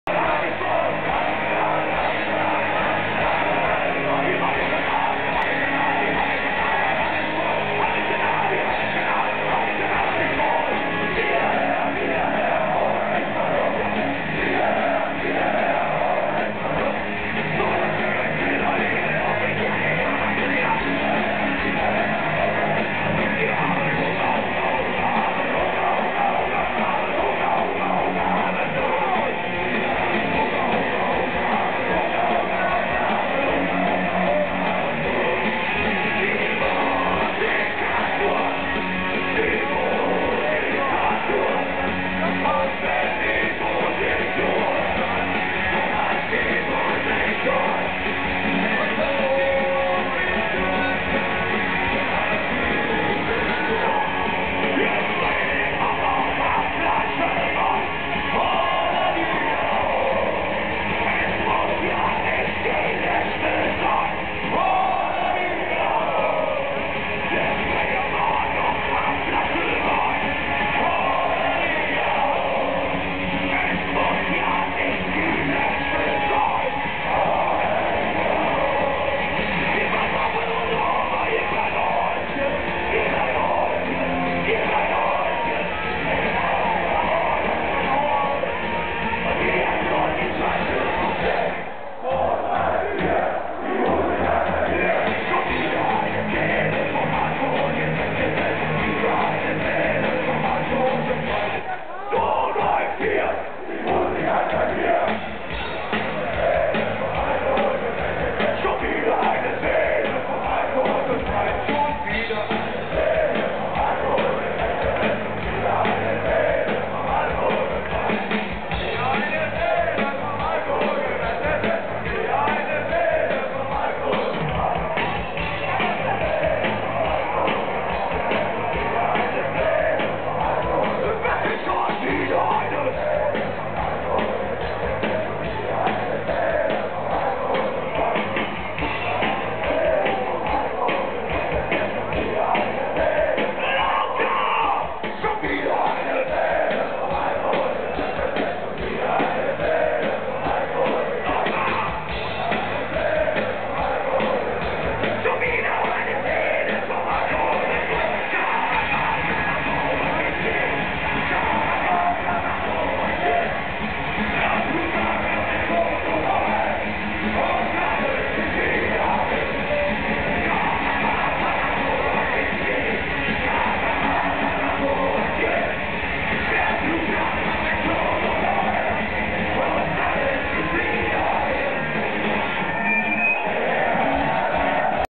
Onkel Tom - Medley Live @ Wacken Open Air 2009
Live video filmed at the Wacken Open Air Festival 2009